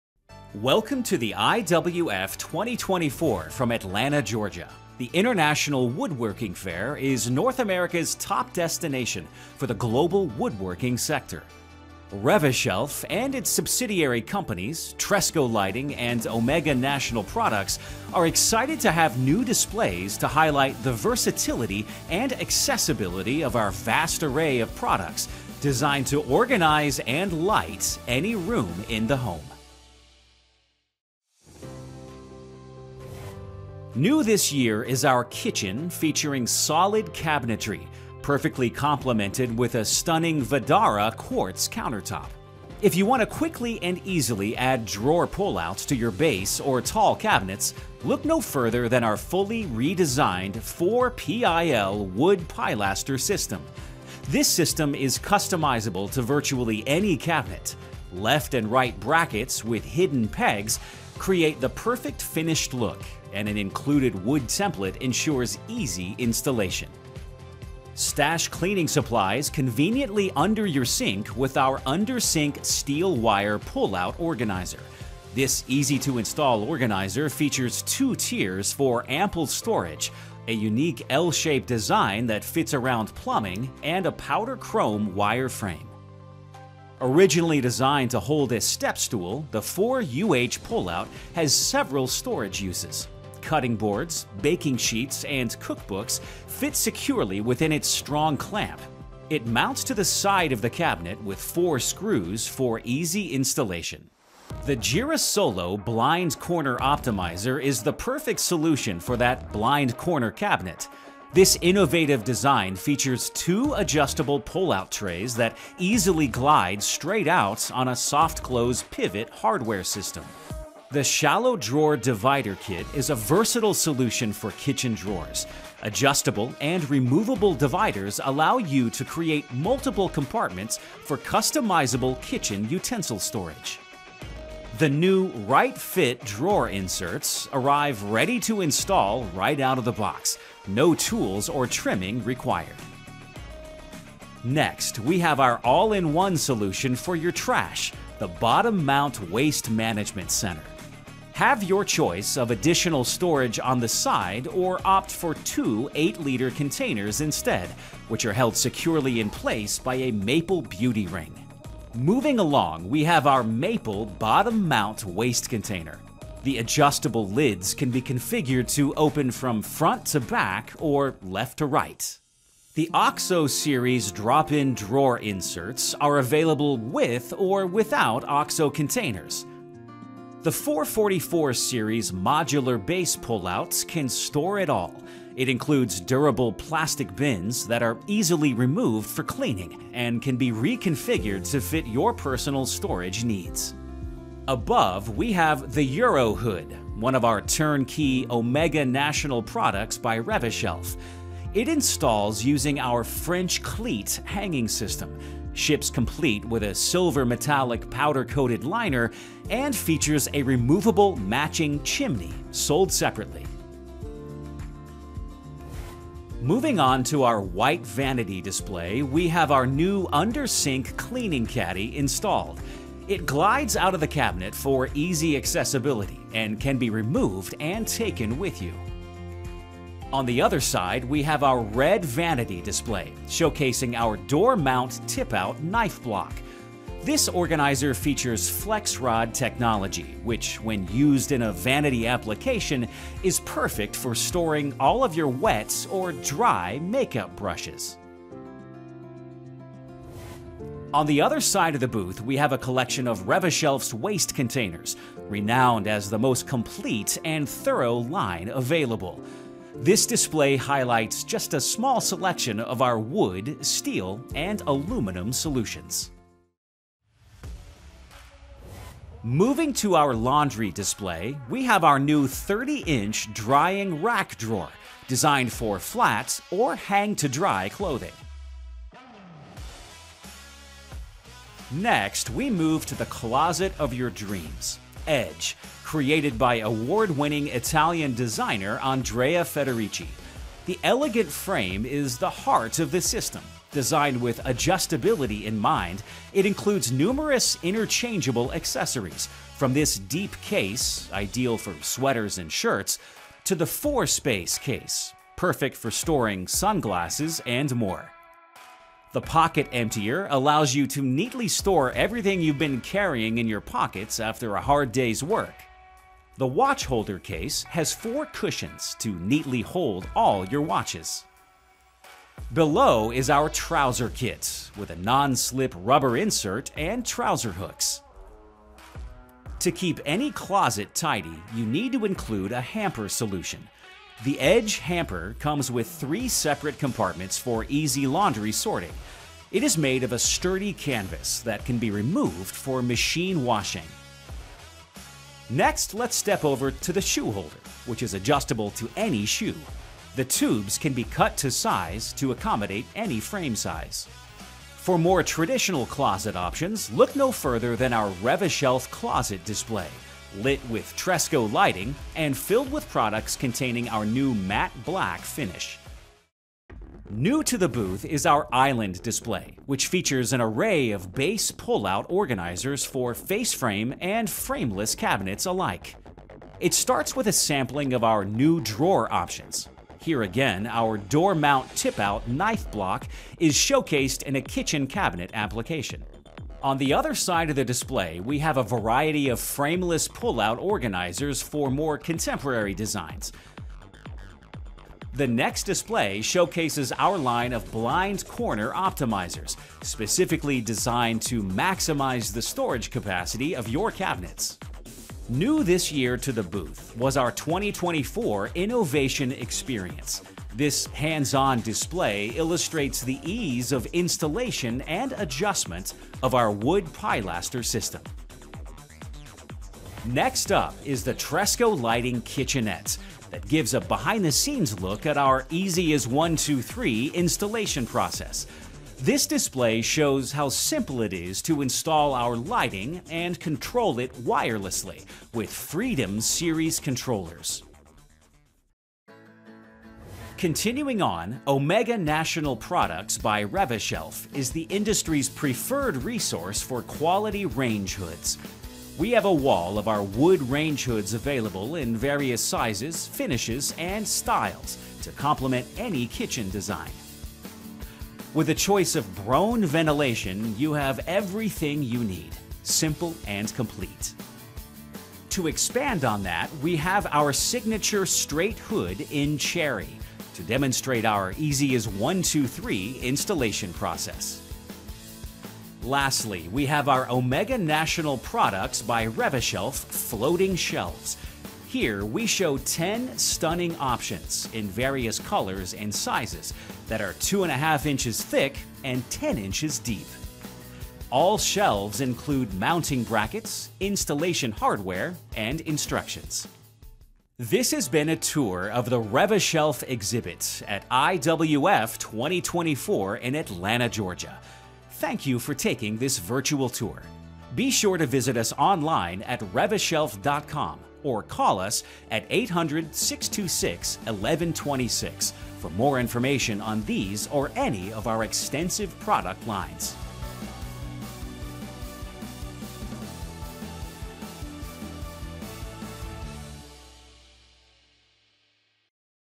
Rev-A-Shelf (IWF 2024) Booth Highlights
A recap of the Rev-A-Shelf booth at the 2024 International Woodworking Fair (IWF) in Atlanta, Georgia.